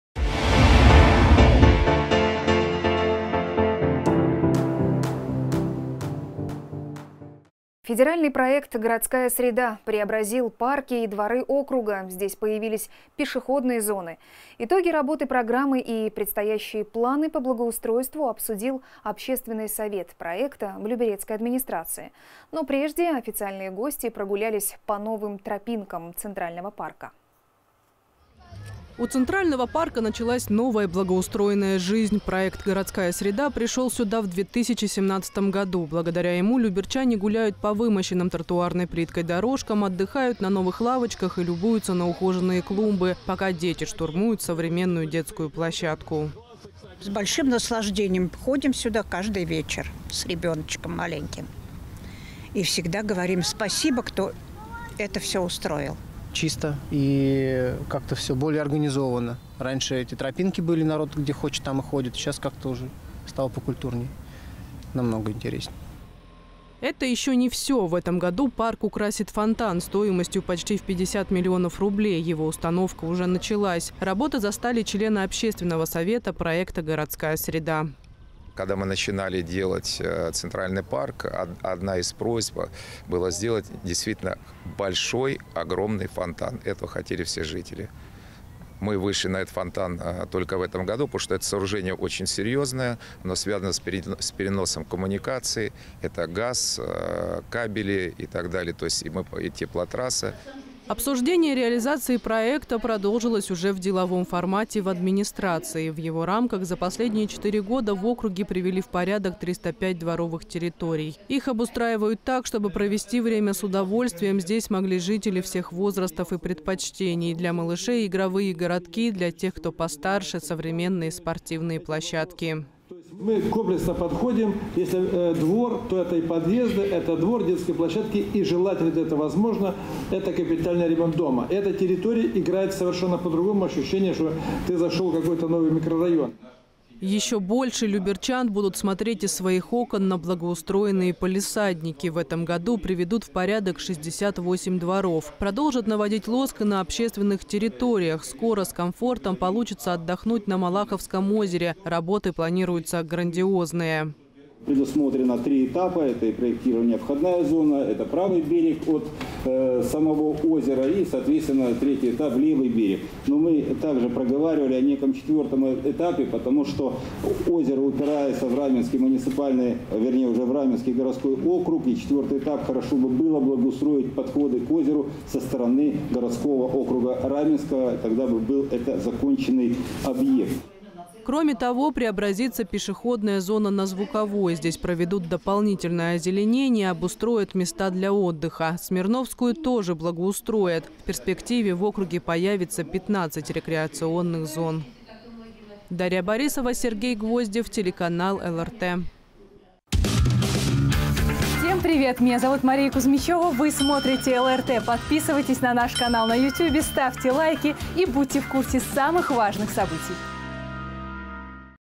"Городская среда" продолжает работать в Люберцах
С приходом в округ федерального проекта  «Городская среда» его парки, пешеходные зоны и дворы со вкусом «принарядились». Итоги работы программы и предстоящие планы по благоустройству в Люберецкой администрации обсудил общественный совет проекта. Но прежде официальные гости прогулялись  по преображенным тропинкам Центрального парка.

У Центрального парка началась новая благоустроенная жизнь. Проект «Городская среда» пришел сюда в 2017 году. Благодаря ему люберчане гуляют по вымощенным тротуарной плиткой дорожкам, отдыхают на новых лавочках и любуются на ухоженные клумбы, пока дети штурмуют современную детскую площадку.

ЛИЛИЯ ВОДЕННИКОВА, ЖИТЕЛЬНИЦА г. ЛЮБЕРЦЫ:
«С большим наслаждением ходим сюда каждый вечер с ребеночком маленьким. И всегда говорим: «Спасибо тем, кто это все устроил».

ВЛАДИМИР ИВАНОВ, ЖИТЕЛЬ г. ЛЮБЕРЦЫ:
«Чисто и как-то все более организованно. Раньше тропинки были – народ где хочет, там и ходит. Сейчас уже как-то стало покультурнее. Намного интереснее».

Это еще не все. В этом году парк украсит фонтан, стоимостью почти в 50 млн. рублей. Его установка уже началась. Работы застали члены общественного совета проекта «Городская среда».

ДМИТРИЙ ШУМСКИЙ, ЗАМЕСТИТЕЛЬ ГЛАВЫ АДМИНИСТРАЦИИ г.о. ЛЮБЕРЦЫ:
«Когда мы начинали делать Центральный парк, одна из просьб была – сделать действительно большой, огромный фонтан. Этого хотели все жители. Мы вышли на этот фонтан только в этом году, потому что это сооружение очень серьезное, оно связано с переносом коммуникаций – это газ, кабели, теплотрасса».

Обсуждение реализации проекта продолжилось уже в деловом формате в администрации. В его рамках за последние 4 года в округе привели в порядок 305 дворовых территорий. Их обустраивают так, чтобы провести время с удовольствием здесь могли жители всех возрастов и предпочтений. Для малышей – игровые городки, для тех, кто постарше – современные спортивные площадки.

ВЛАДИМИР РУЖИЦКИЙ, ГЛАВА г.о. ЛЮБЕРЦЫ:
«Мы комплексно подходим. Если двор, то это и подъезд, и двор, и детские площадки, и, желательно, где это возможно, это капитальный ремонт дома. Эти территории играют совершенно по-другому. Ощущение, что ты зашел в какой-то новый микрорайон».

Еще больше люберчан будут смотреть из своих окон на благоустроенные палисадники. В этом году приведут в порядок 68 дворов. Продолжат наводить лоск и на общественных территориях. Скоро с комфортом получится отдохнуть на Малаховском озере. Работы планируются грандиозные.

ИГОРЬ КОХАНЫЙ, РЕГИОНАЛЬНЫЙ КООРДИНАТОР ФЕДЕРАЛЬНОГО ПРОЕКТА «ГОРОДСКАЯ СРЕДА»:
«Предусмотрено 3 этапа – это проектирование входной зоны, это правый берег от самого озера. И, соответственно, 3 этап – левый берег. Но мы также проговаривали о некоем 4 этапе, потому что озеро упирается в Раменский городской округ и на 4 этапе хорошо бы было обустроить подходы к озеру со стороны городского округа Раменский. Тогда бы это был законченный объект».

Кроме того, преобразится пешеходная зона на Звуковой. Здесь проведут дополнительное озеленение, обустроят места для отдыха. Смирновскую тоже благоустроят. В перспективе в округе появится 15 рекреационных зон.

Дарья Борисова, Сергей Гвоздев